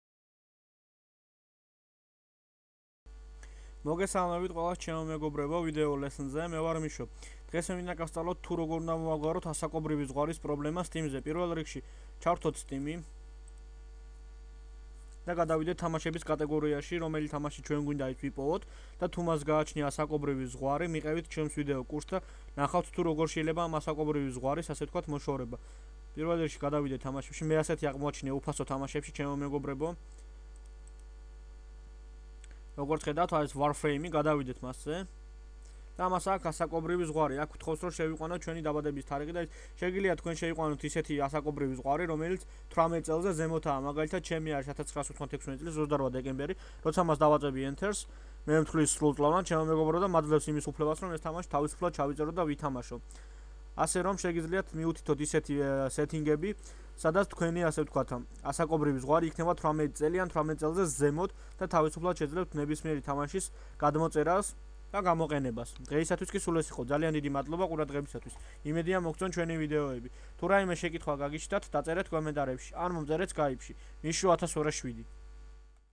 როგორ მოვიშორო ასაკობრივი ზღვარი Steam-ზე
ამ ვიდეო გაკვეთილით მე მინდა გასწავლოთ სტიმზე ასაკობრივი ბარიერის მოშორება (ეს ასაკობრივი ბარიერი გააჩნია ძალიან სასტიკი სიუჟეტების მქონე თამაშებს ან ძალიან ჰორორ მომენტების მქონე თამაშებს) ხოლო ჩემი ვიდეო გაკვეთილით ძალიან მარტივად შეძებთ ასაკობრივი ზღვრის მოშორებას